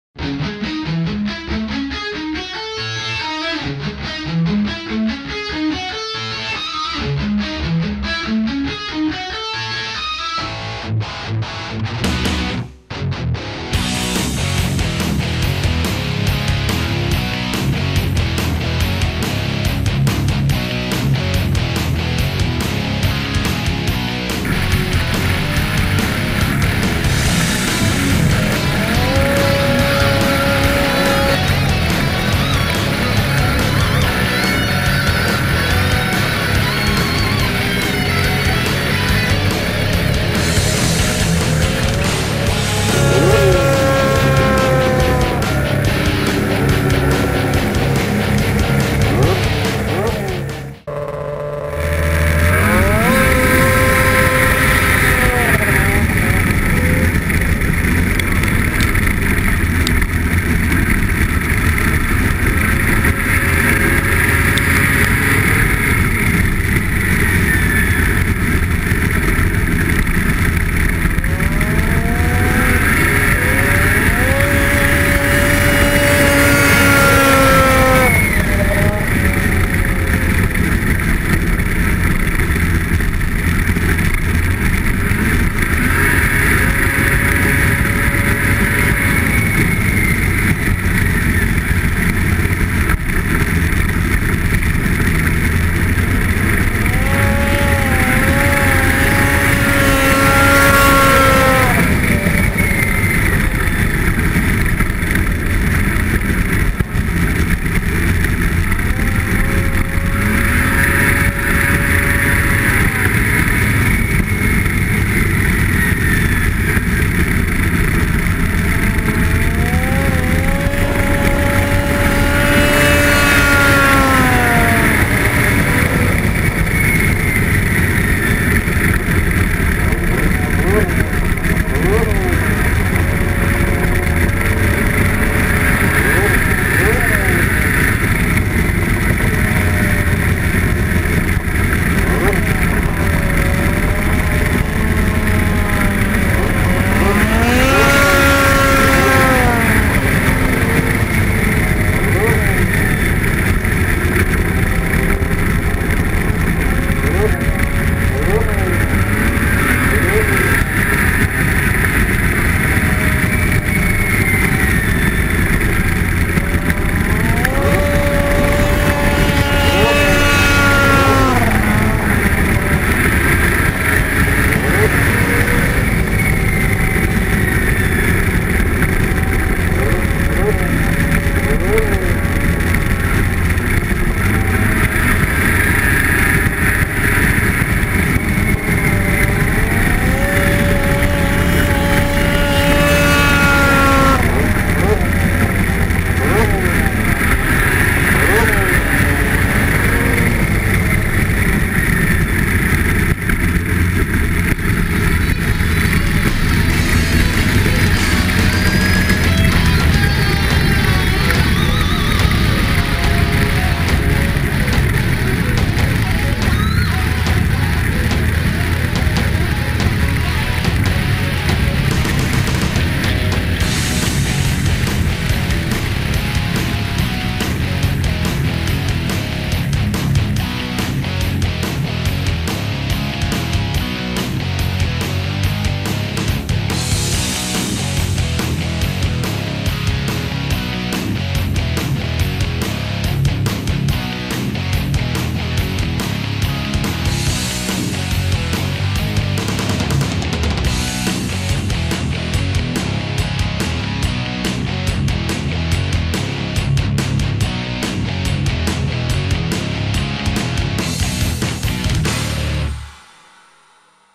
GoPro HD Sportsman Snowmobile Ice Racing
Thanks to Jim & Calen of Wilbur Family Racing for hosting my GoPro HD cam on their Polaris 500XC sled. This Ice Racing Alaska event was held Jan 9, 2011 on Big Lake, Alaska  oval track. Jims'  son, Calen, running in the Sportsman/Junior class does all the riding in this vid running against Ryan Goldfuss. I cut short the rear facing scenes and concentrated on the forward interesting look since it offered more to the riders who can review the vid and see their track handling. The GoPro was in a full case to protect it from snow & ice so audio is really bad but the crackling noise is from the skis on the ice. I layered sound from my hand held over it to make it a little better.